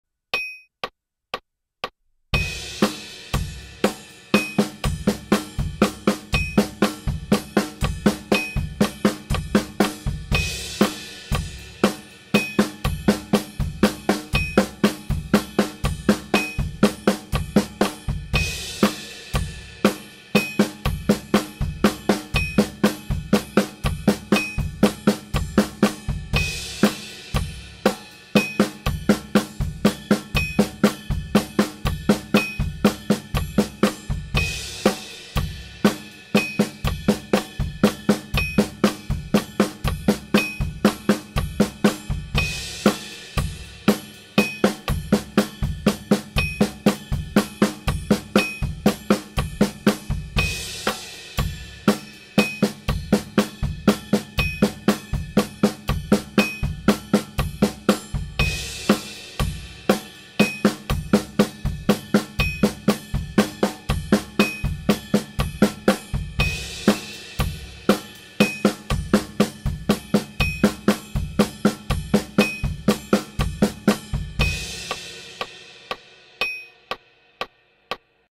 8分音符3つ割りの手足コンビネーション（♩＝120）/ 2023.09.19
左手と右足で「LLK」をしながら
右手は4分音符のサイクルをキープ